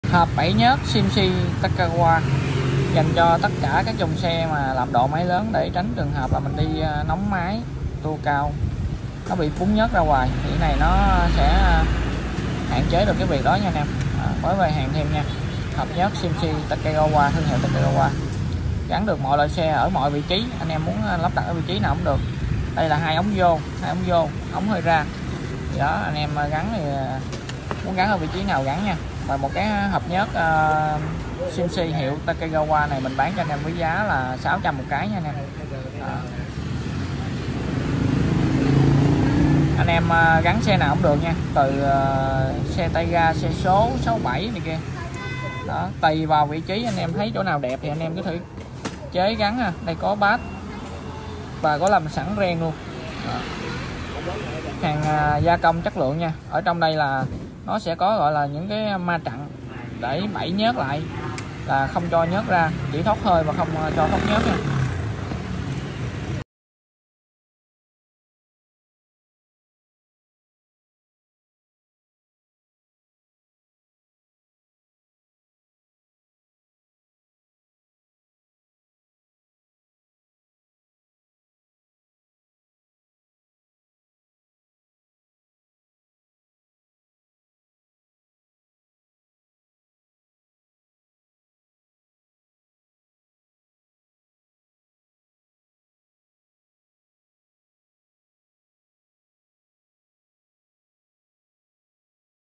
🆙 600k 🤘HỘP BẨY NHỚT CNC TAKE SIZE 88 x 72 CHO 67 CD EX DREAM WAVE MỌI LOẠI XE
🤘HỘP BẨY NHỚT CNC TAKE SIZE 88 x 72 CHO 67 CD EX DREAM WAVE MỌI LOẠI XE

🩸 GIÁ: 600k / HỘP

✅ Rất hữu dụng cho xe có độ máy ở mọi cấu hình và mọi thể loại từ 201m đến sân tròn hay 2 cua , đặc biệt với xe touring đường dài thì đây là một sản phẩm thiết yếu.

✅ Hầu hết mọi xe nâng cấp động cơ đều phải cần nâng cấp thêm thông hơi - tuy nhiên vấn đề ra nhớt ở thông hơi trong quá trình vận hành làm hao hụt nhớt là điều mà rất nhiều anh em hay gặp .

✅Với bình thông hơi có tích hợp bẫy nhớt Sum racing thì có thể giải quyết ngay vấn đề hao hụt nhớt ra đường thông hơi mà vẫn thoáng máy mát máy.

✅ Sản phẩm có loại 2 val hơi và 3 val hơi dành cho anh em đao to búa lớn vẫn đủ dùng. Size : NHỎ VÀ LỚN

✅ Được làm từ nhôm CNC cực kì đẹp mắt và thẩm mĩ.

+ Agribank, CN Bình Tân, HCM
+ Đông Á Bank, CN Trường Chinh , HCM
+ Sacombank , CN Hốc Môn, HCM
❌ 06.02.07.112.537